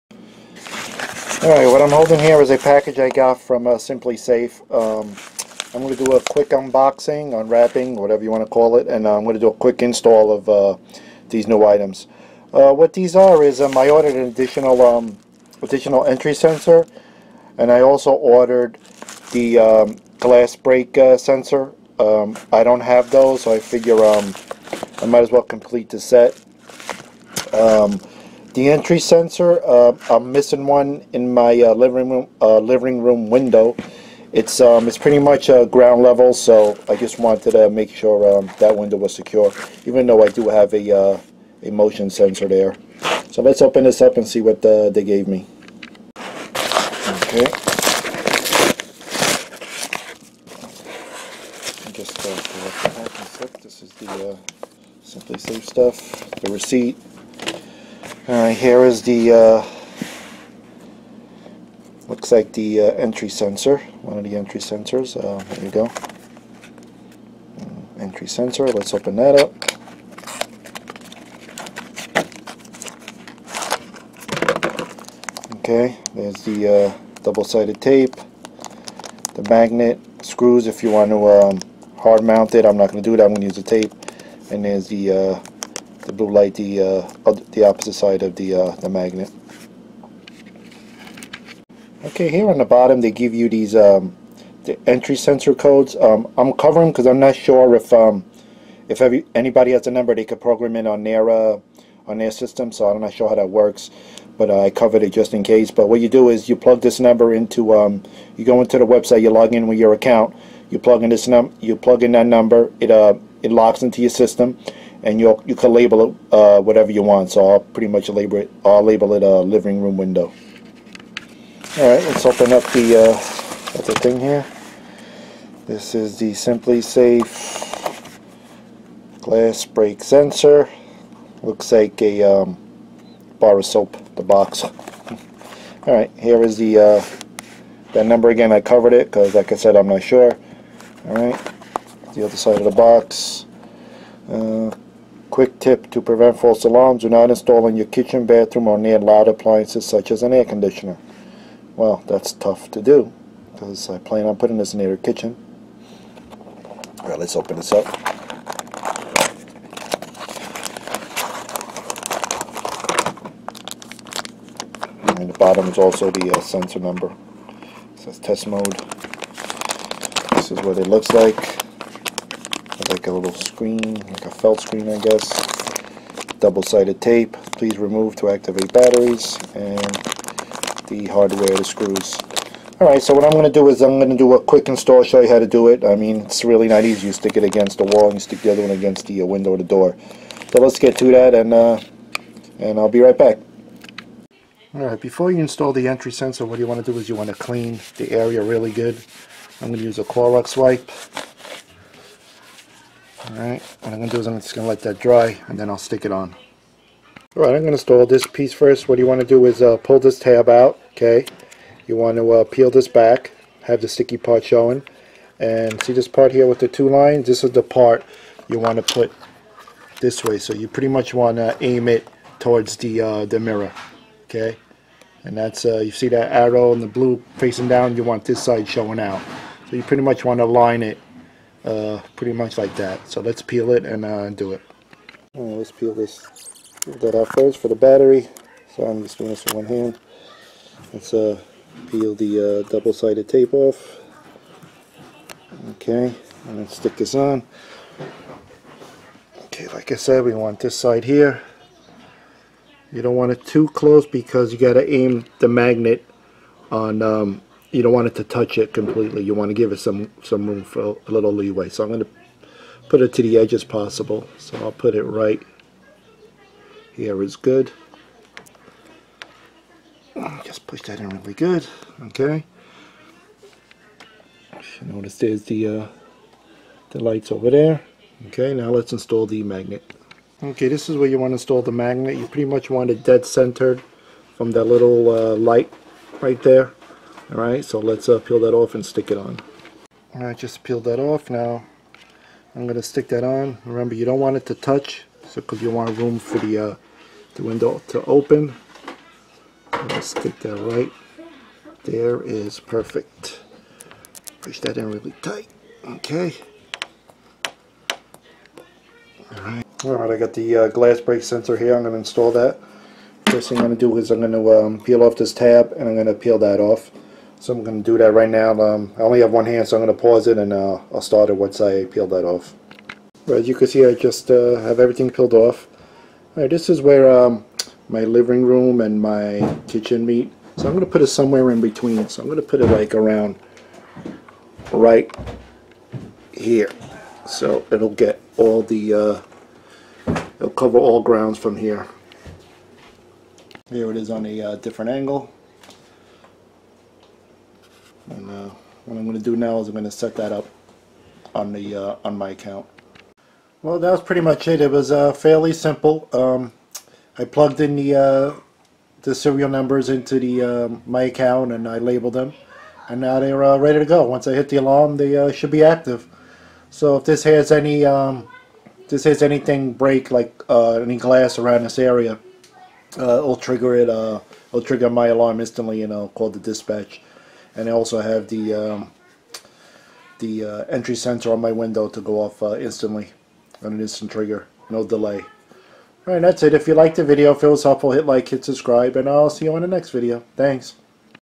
Install Simplisafe Entry sensor and Glass Break Sensor
Simple DIY on how to install sensors